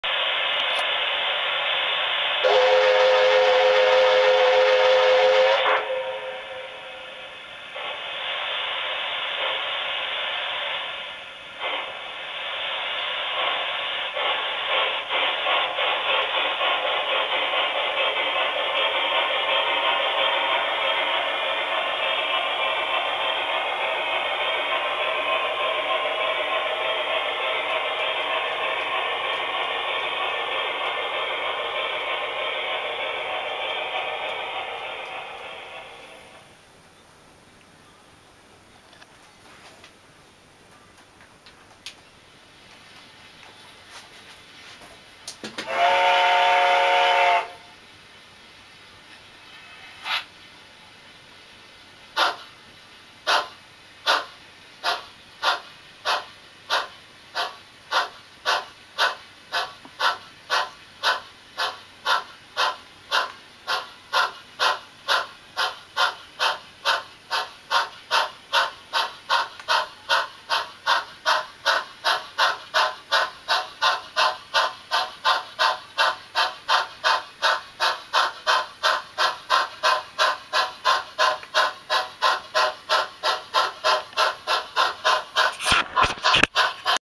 After Market Sound Decoder Comparison in a C36. MVI_1233.AVI
A comparison between two "After Market" sound decoders that we can install in our existing fleet. The QSI Solution's Revolution in 3642 and a Soundtraxx Tsunami in 3622. These are identical locos, an Austrains C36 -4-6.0, using the same Tony's High Bass HB1.06 speaker installation and using the Tender as the enclosure.
The Revoltion in 3642 has all the same features as the QSI equipped locos from Eureka and Austrains e.g. Heavy Load etc, a C35 Whistle & 6 Function outputs for Headlights, Markers, Firebox Flicker & Cab lights..
The Tsunami has a 7 band Audio Equaliser, selected in the video at "Flat", where the user can enhance the sound of the decoder. For the Whistle, you select one of the 7 available whistles. Use either the Light or K Class Tsunamis. I prefer the selection in the Light. The TSU-1000 has 4 Function outputs while the smaller TSU-750 Micro has 2 functions.